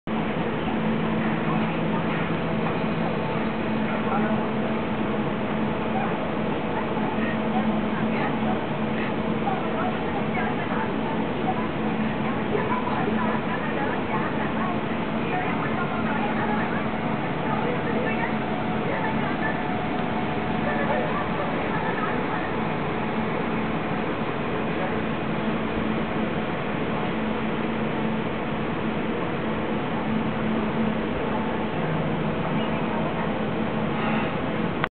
The Ops Rm @ Hill St
A peek into my office - 360 degrees
before the move to fusionopolis in May 2008